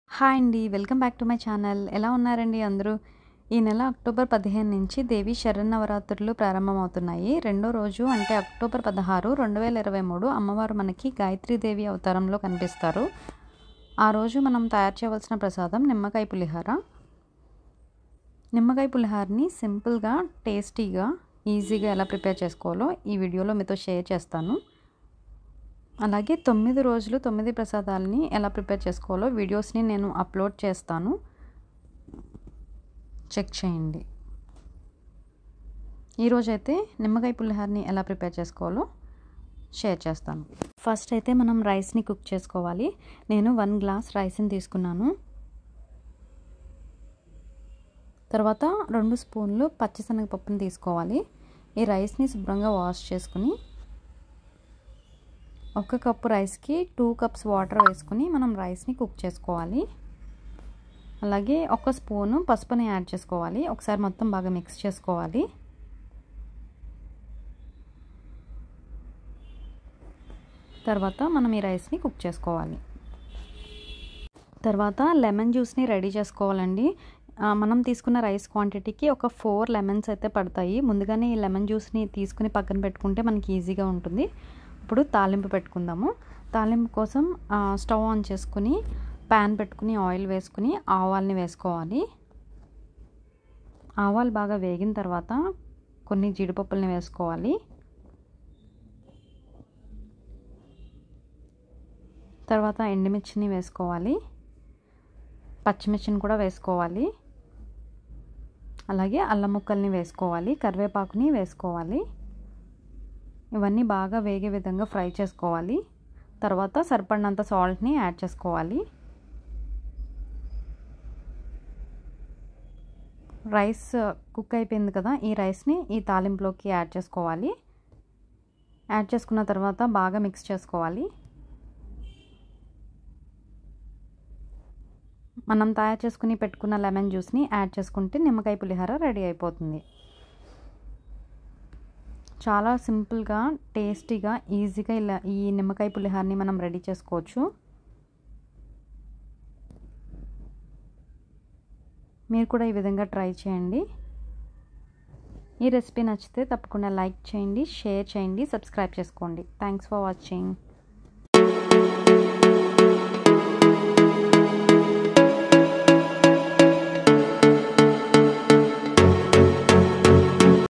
నవరాత్రిల్లో అమ్మవారికి రెండవ రోజు ప్రాసాదం నిమ్మకాయపులిహోర Dussehra prasadam recipes in telugu
navaratri day 2 ప్రాసాదం నిమ్మకాయ పులిహోర
Dussehra recipes in telugu
navaratri prasadalu.